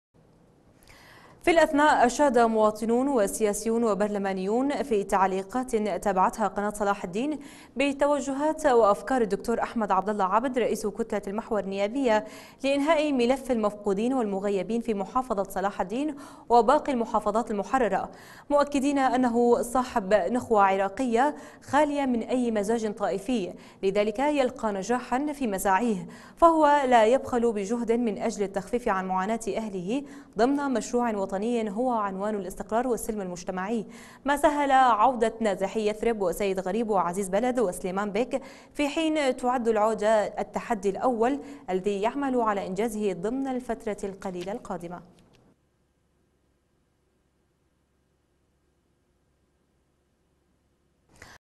اشادة واسعة بتوجهات الدكتور ابو مازن لانهاء ملف المغيبين
الاستقرار والسلم المجتمعي

في الاثناء اشاد مواطنون وسياسيون وبرلمانيون في تعليقات تابعتها قناة صلاح الدين , بتوجهات وافكار الدكتور احمد عبدالله عبد رئيس كتلة المحور النيابية لإنهاء ملف المفقودين والمغيبين في محافظة صلاح الدين وباقي المحافظات المحررة , مؤكدين انه صاحب نخوة عراقية خالية من أي مزاج طائفي لذلك يلقى نجاحا في مساعيه , فهو لا يبخل بجهد من اجل التخفيف عن معاناة اهله ضمن مشروع وطني هو عنوان الاستقرار والسلم المجتمعي ما سهل عودة نازحي يثرب وسيد غريب وعزيز بلد وسليمان بيك في حين تٌعد العوجة التحدي الاول الذي يعمل على انجازه ضمن الفترة القليلة القادمة .